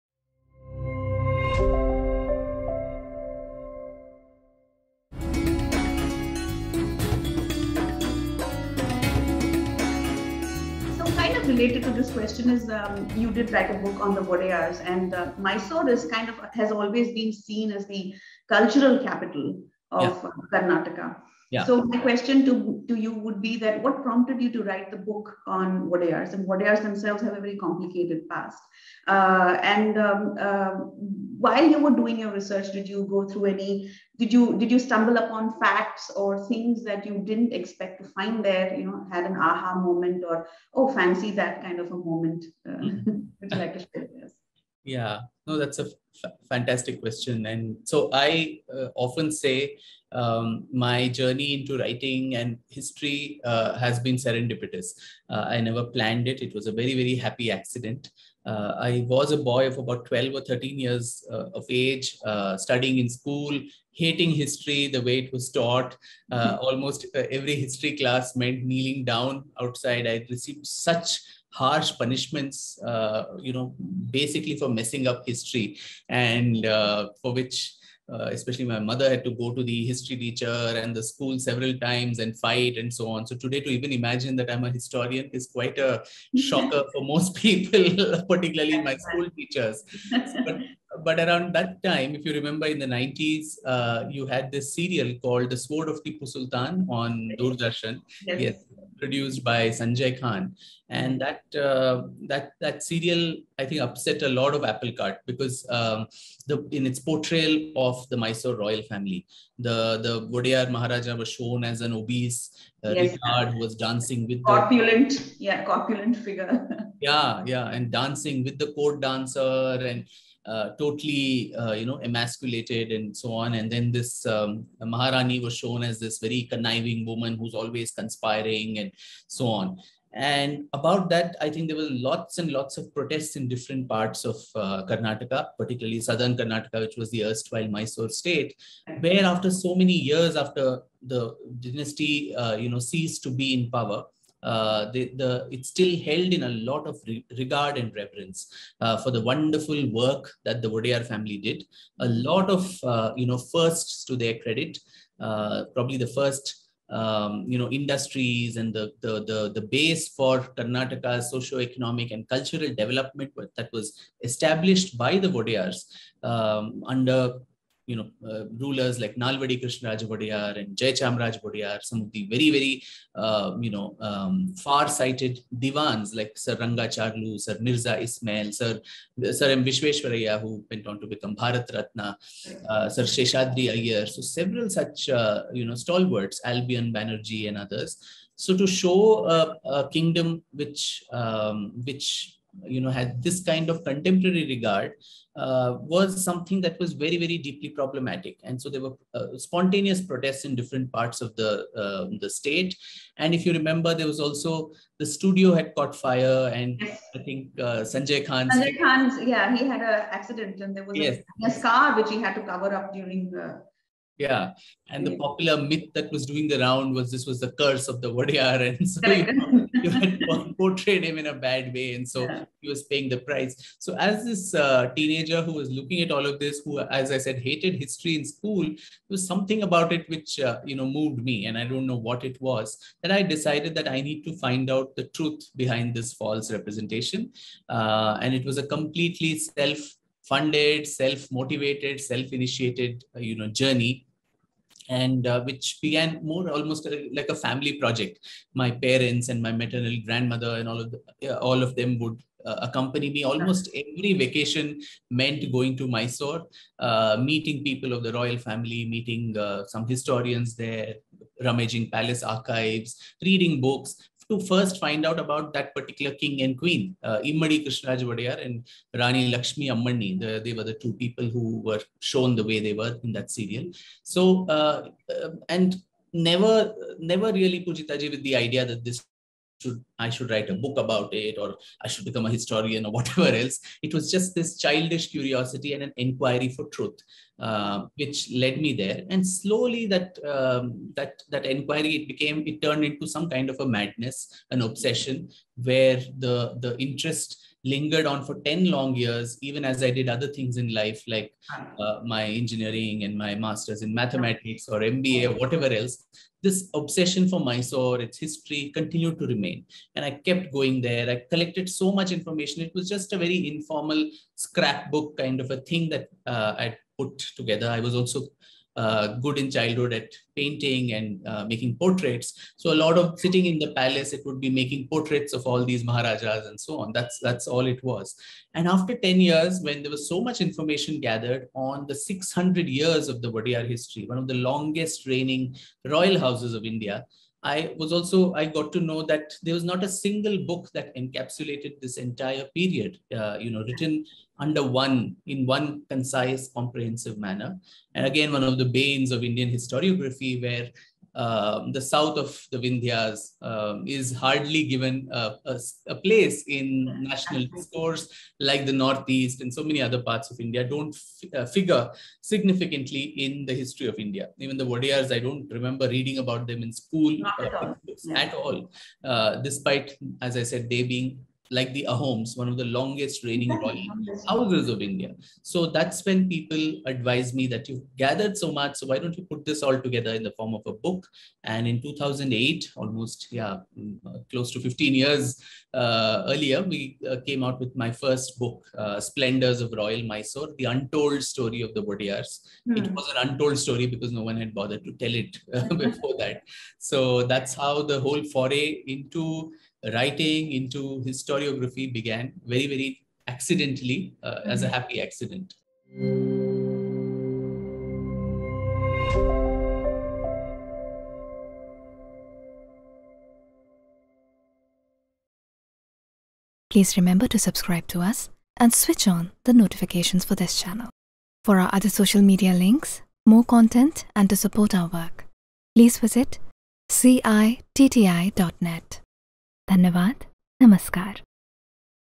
"They glorified Tipu Sultan, erased Hindu history." Vikram Sampath
They glorified a tyrannical mass murderer in Tipu Sultan, whilst wilfully debasing Mysore's 600-year-rich Wodeyar Dynasty: pioneering historian, Vikram Sampath